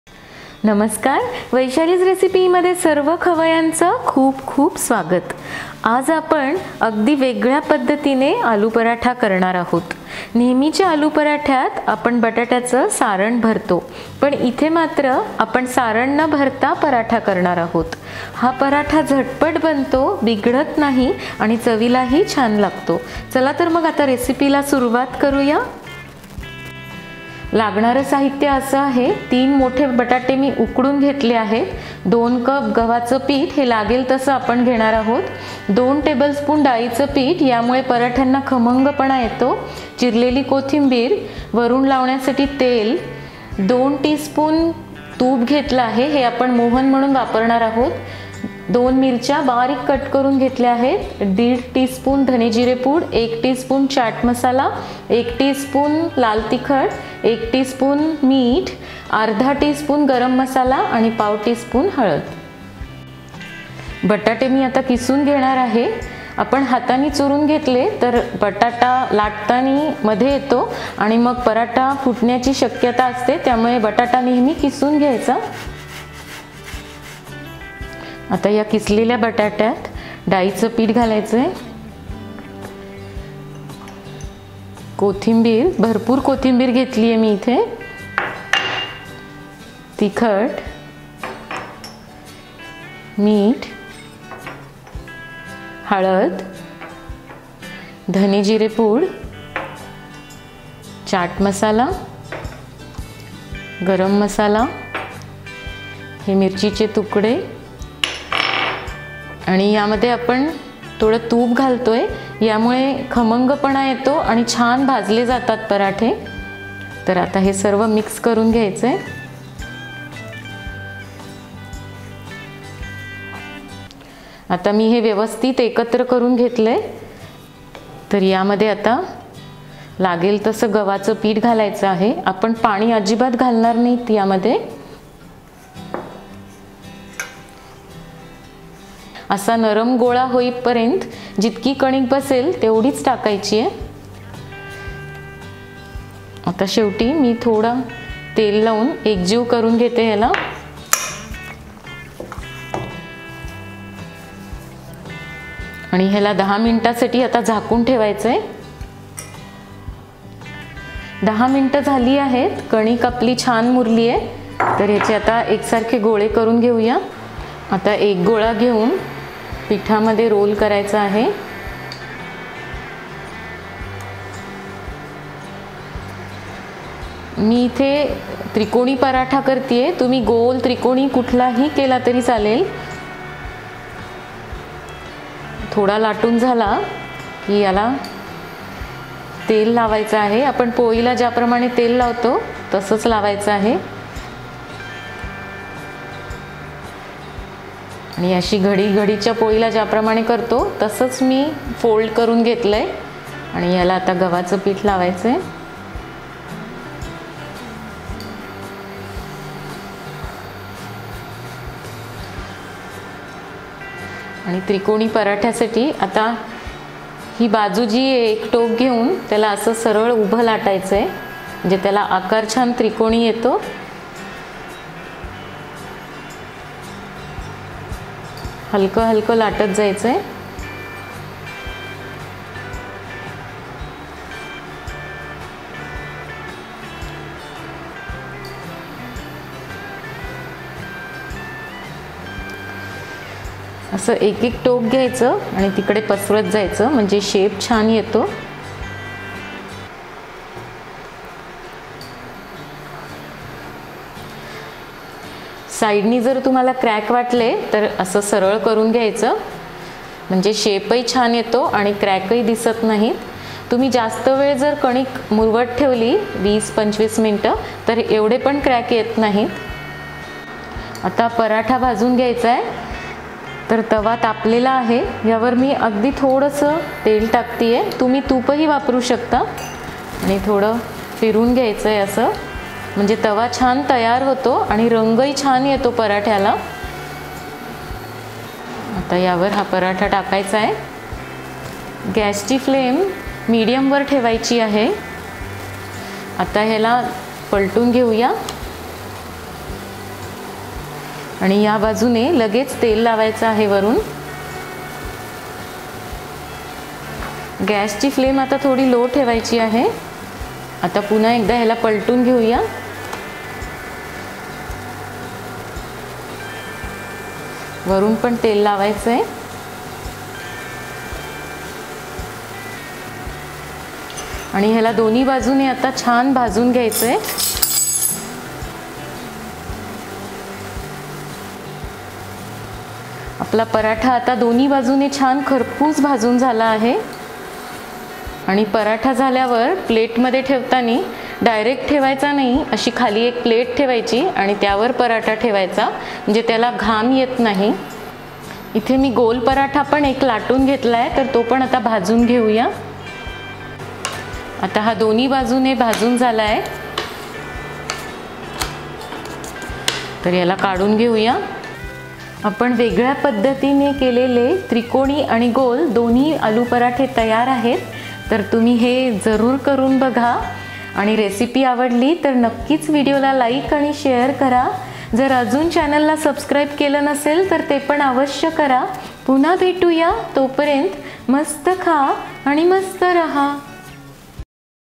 असा बनवा आलू पराठा "न फाटता,सारण तयार न करता" झटपट होणारा/Easy & Quick Aloo paratha recipe
साहित्य :
उकडलेले बटाटे ३
डाळी चं पीठ २ चमचे
तिखट
मीठ
हळद
धने जिरे पूड
गरम मसाला
चाट मसाला
हिरवी मिरची
कोथिंबीर

Ingredients :
Boiled Potatoes 3
Bengal gram flour 2 Tbsp
Red chilli powder
Salt
Turmeric powder
Coriander & Cumin powder
Garam masala
Chat masala
Green chilli Coriander